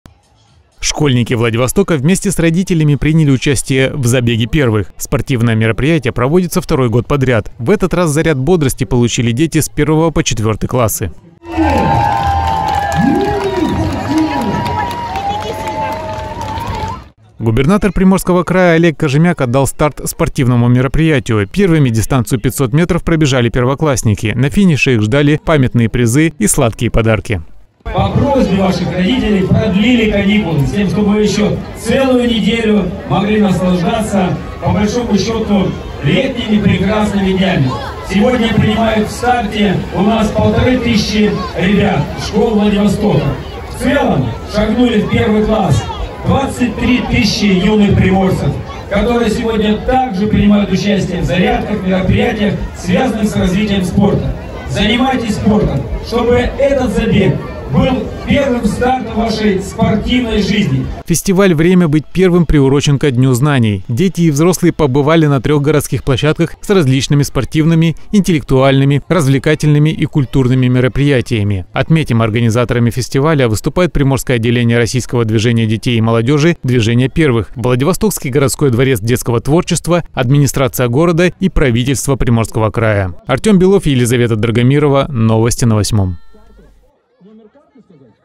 Забег для первоклашек состоялся в столице Приморья.
Школьники Владивостока вместе с родителями приняли участие в «Забеге первых». Спортивное мероприятие проводится второй год подряд. В этот раз заряд бодрости получили дети с 1 по 4 классы.